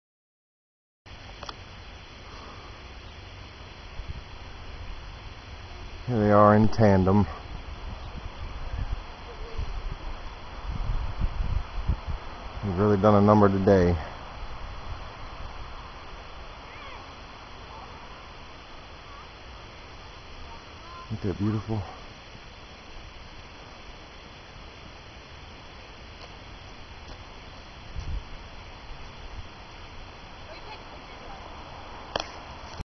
IMGP1548.AVI
Saturday, November 19,2011 I went outside at 10:00am to do some yard work, as I always do when I step outdoors, I looked up, not a cloud in the sky, what a beautiful sight! As i started my work I began to notice the planes overhead leaving behind their plumes. In a very short period of time the sky was almost completely saturated with that ugly silvery haze. I happen to catch these 2 aircraft flying in formation across the sky. The onslaught continued for the rest of the day. I checked for commercial aircraft in our airspace and guess what? There were 2 on flight aware! hmmm. I also checked atmospheric conditions, 2 readings taken and guess what? conditions were not favorable for "contrails to form, yet alone to form, persist and spread like a blanket. I don't know guys you tell me. (by the way, in the video when I say it's a beautiful thing, I was being sarcastic)